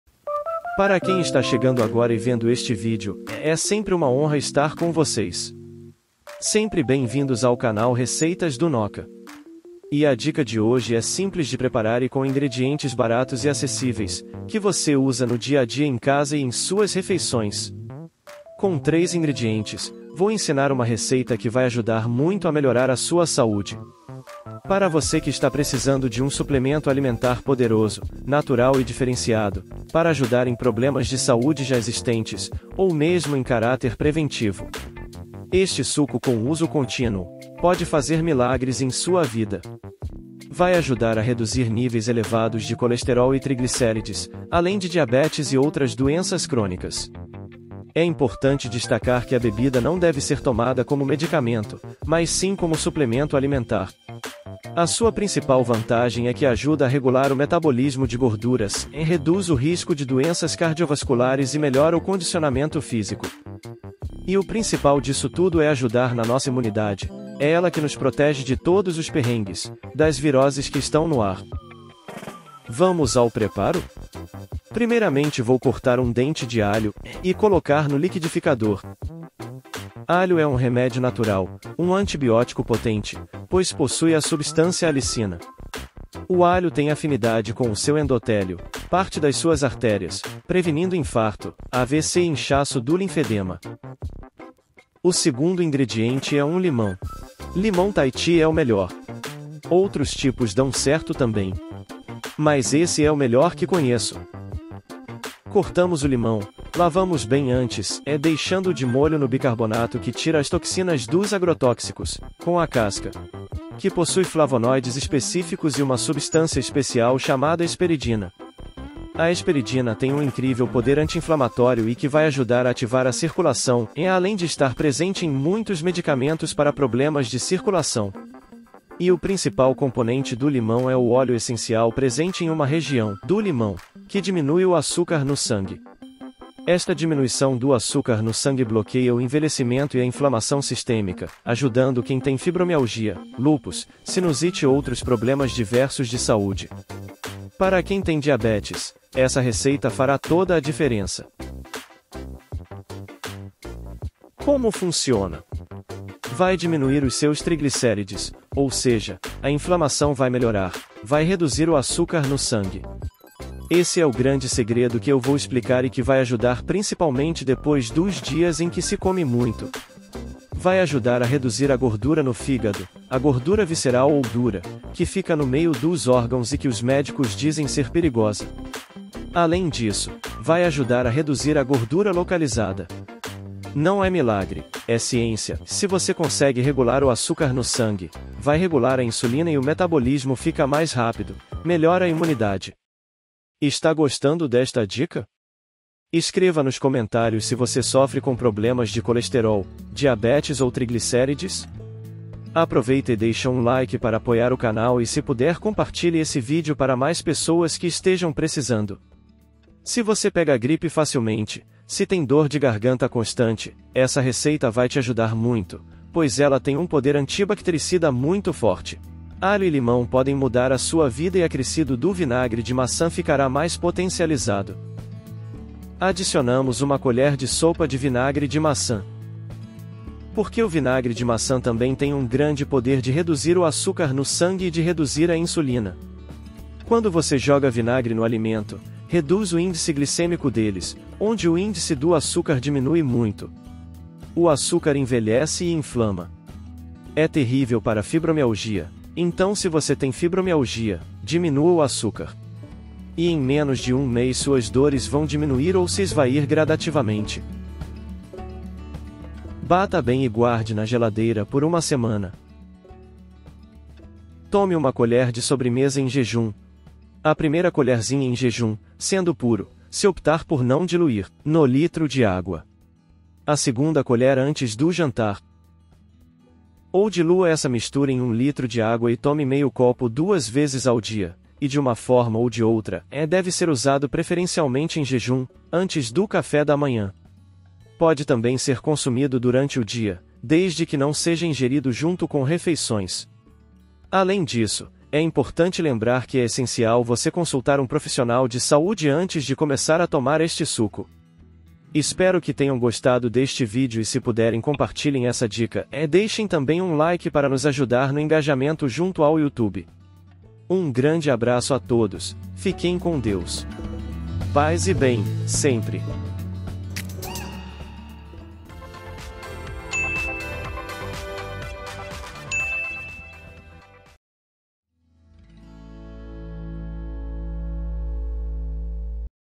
Dica MELHOR do MUNDO para BAIXAR seus TRIGLICERÍDEOS e COLESTEROL.
DICA para LIMPAR seu FÍGADO, reduzir COLESTEROL, TRIGLICERÍDEOS e melhorar os índices de AÇUCAR de seu sangue. Vale a pena conferir neste vídeo. É simples e fácil de fazer.

ஜ۩۞۩ஜ
▓▓▓▒▒▒░░░  Sejam todos super bem-vindos!

GOSTARIA DE NOS ENVIAR UM MIMO?